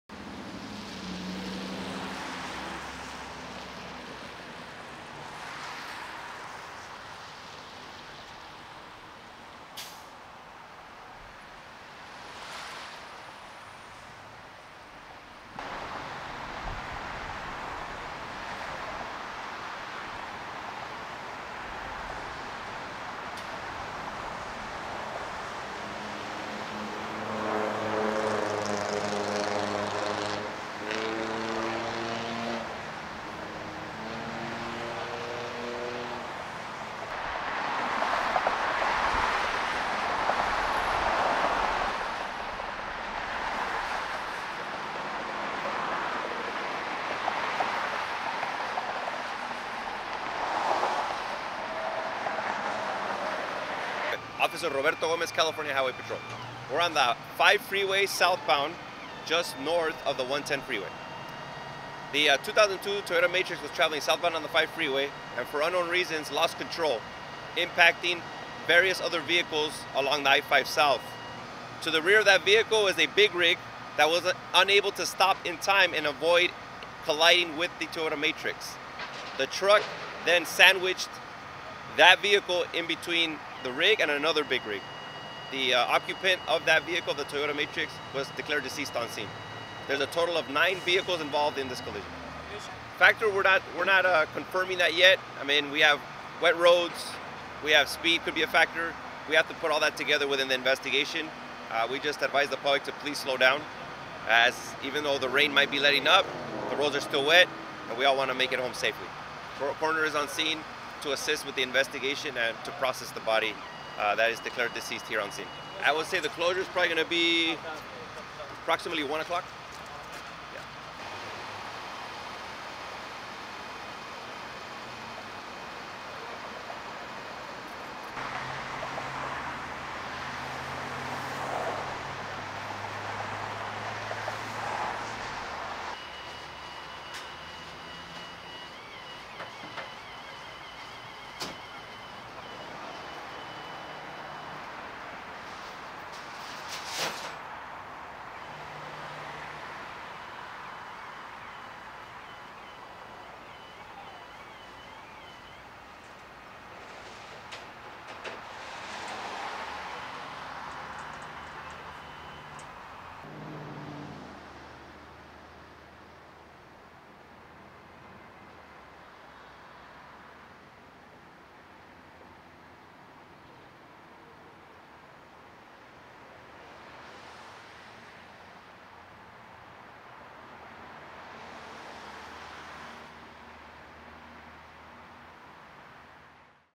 Fatal Collision Involving 9 Vehicles
Traffic Fatality, 1st Ld Person Killed, Three Hospitalized From Multi-Vehicle Crash Near Dodger Stadium Eds: ADDS ages, genders of hospitalized. LOS ANGELES (CNS) - A person was killed and paramedics rushed three people to area hospitals from a traffic crash today involving at least a half-dozen vehicles -- including two big rigs -- on the Golden State (5) Freeway near Dodger Stadium, in Elysian Park. A 67-year-old woman, a 29-year-old woman and an 11-year-old girl were transported to area hospitals. Their conditions were unknown, said Nicholas Prange of the Los Angeles Fire Department. Four other people injured in the crash declined transport, said Prange, adding that ``one patient who was transported required extrication by firefighters with hydraulic rescue tools." Firefighters and paramedics were dispatched at 8:07 a.m. to the southbound freeway at Stadium Way, said Nicholas Prange of the Los Angeles Fire Department. A representative from the coroner's office was called out to the scene at 8:55 a.m., according to the California Highway Patrol . Traffic was snarled on the southbound 5 and motorists were encouraged to find another route. The cause of the crash was not immediately known.